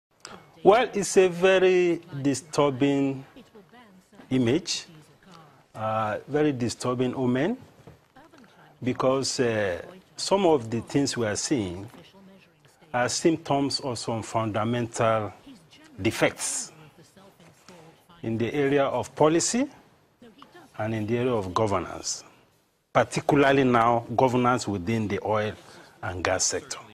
UNBELIEVABLE; Edo State Governor was caught on sex scandal through camera in a Hotel
UNBELIEVABLE; Edo State Governor was caught on sex scandal through camera